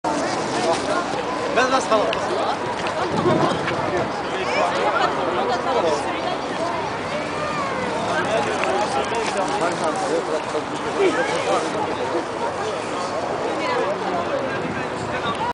Eiffel Tower
We just arrived in paris and we went to see the eiffel tower